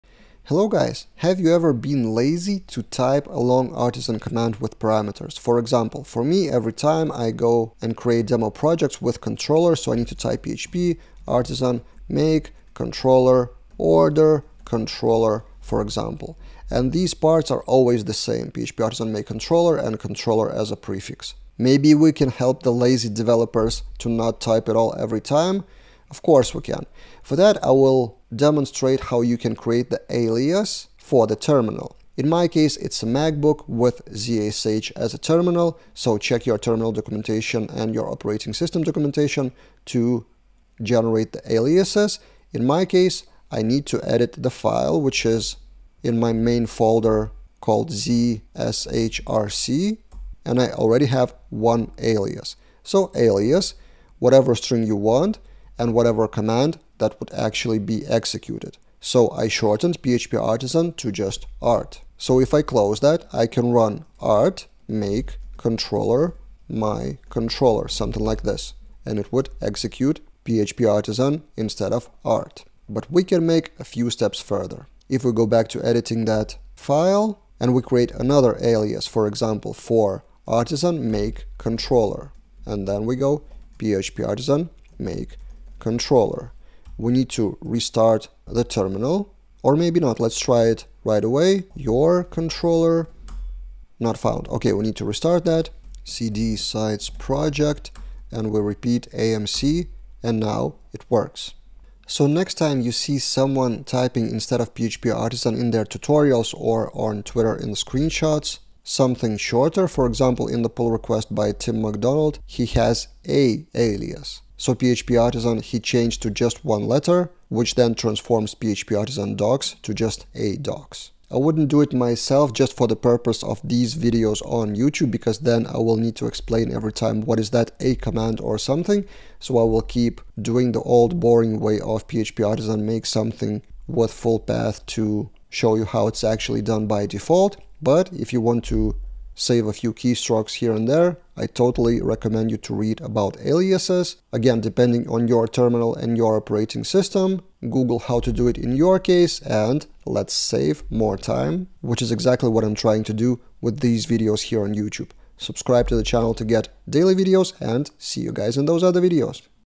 Terminal Aliases: Shorten "php artisan" to Whatever
I've seen people optimize their typing experience by assigning Terminal aliases to their most used Artisan commands. Let me show you.